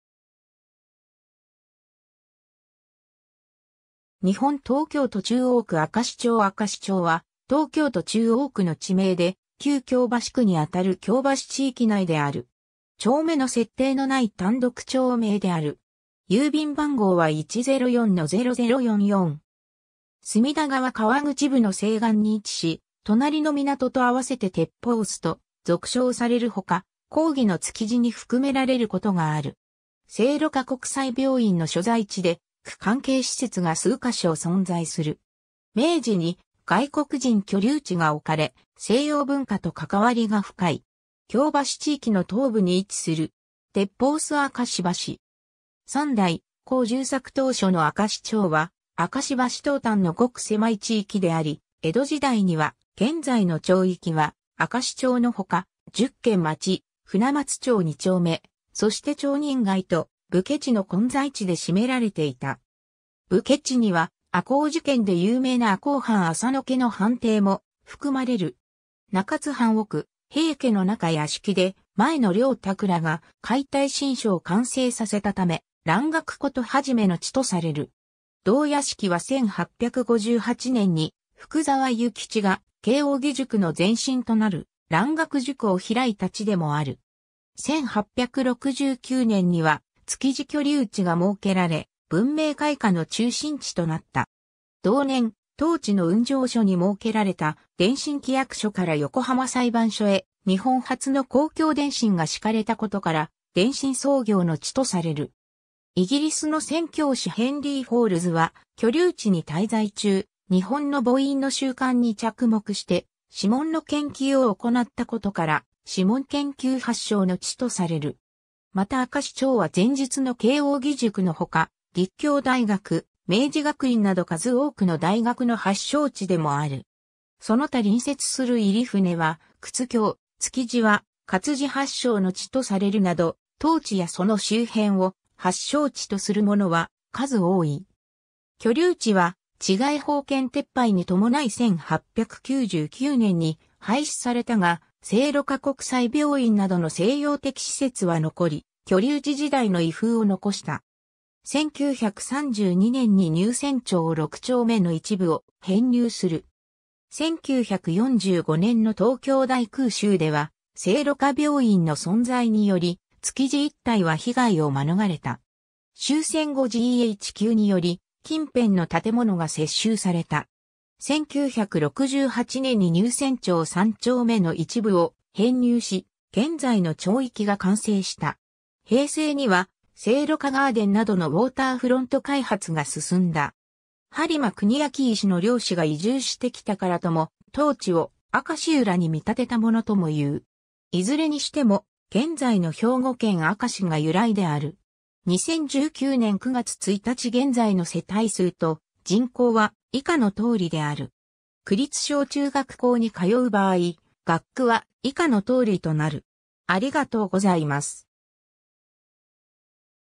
明石町 (東京都中央区)
日本 」 東京都 」 中央区 」 明石町 明石町（あかしちょう）は、東京都中央区の地名で、旧京橋区に当たる京橋地域内である。
 隅田川河口部の西岸に位置し、隣の湊と合わせて鉄砲洲と俗称されるほか、広義の築地に含められることがある。
聖路加国際病院の所在地で、区関係施設が数か所存在する。
明治に外国人居留地が置かれ、西洋文化と関わりが深い。
 京橋地域の東部に位置する。
 鉄砲洲明石橋。
3代広重作 当初の明石町は明石橋東端の極狭い地域であり、江戸時代には現在の町域は明石町の他、十軒町、船松町二丁目、そして町人街と武家地の混在地で占められていた。
武家地には赤穂事件で有名な赤穂藩浅野家の藩邸も含まれる。
 中津藩奥平家の中屋敷で前野良沢らが解体新書を完成させたため、蘭学事始の地とされる。
同屋敷は1858年（安政5年）に福澤諭吉が慶應義塾の前身となる蘭学塾を開いた地でもある。
 1869年（明治2年）には築地居留地が設けられ、文明開化の中心地となった。
同年、当地の運上所（現：東京税関）に設けられた電信機役所から横浜裁判所（現：横浜地方裁判所）へ日本初の公共電信が敷かれたことから、電信創業の地とされる。
 イギリスの宣教師ヘンリー・フォールズは居留地に滞在中、日本の拇印の習慣に着目して指紋の研究を行ったことから、指紋研究発祥の地とされる。
 また明石町は前述の慶應義塾の他、立教大学、明治学院など数多くの大学の発祥地でもある。
その他隣接する入船は靴業、築地は活字発祥の地とされるなど、当地やその周辺を発祥地とするものは数多い。
 居留地は治外法権撤廃に伴い1899年（明治32年）に廃止されたが、聖路加国際病院などの西洋的施設は残り、居留地時代の遺風を残した。
1932年（昭和7年）に入船町六丁目の一部を編入する。
 1945年（昭和20年）の東京大空襲では、聖路加病院の存在により築地一帯は被害を免れた。
終戦後GHQにより近辺の建物が接収された。
 1968年（昭和41年）に入船町三丁目の一部を編入し、現在の町域が完成した。
平成には聖路加ガーデンなどのウォーターフロント開発が進んだ。
 播磨国明石の漁師が移住してきたからとも、当地を明石浦に見立てたものともいう。
いずれにしても、現在の兵庫県明石が由来である。
 2019年（令和元年）9月1日現在の世帯数と人口は以下の通りである。